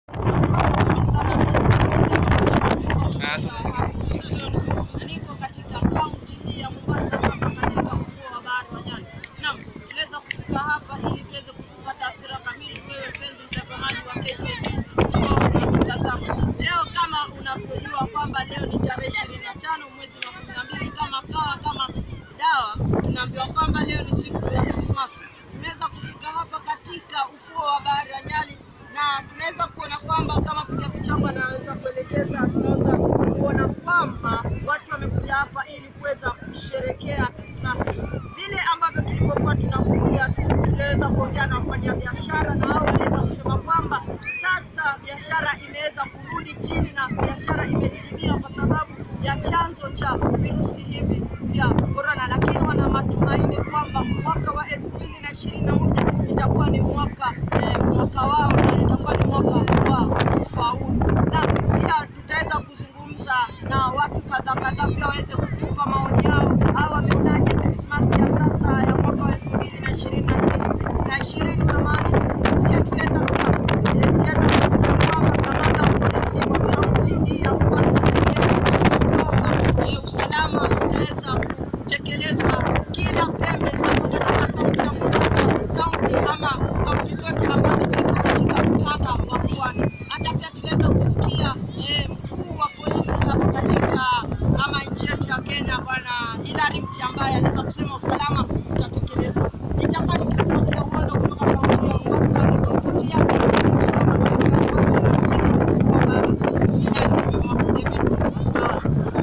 SHAMRA SHAMRA ZA KRISI MOMBASA.
Christmas katika kaunti ya Mombasa imeshika mno.Wakazi wa ukanda wa pwani wakutana kusherehekea christmas pamoja.Tafadhali tazama video hii ili upate uhondo yaliyojiri Nyali beach.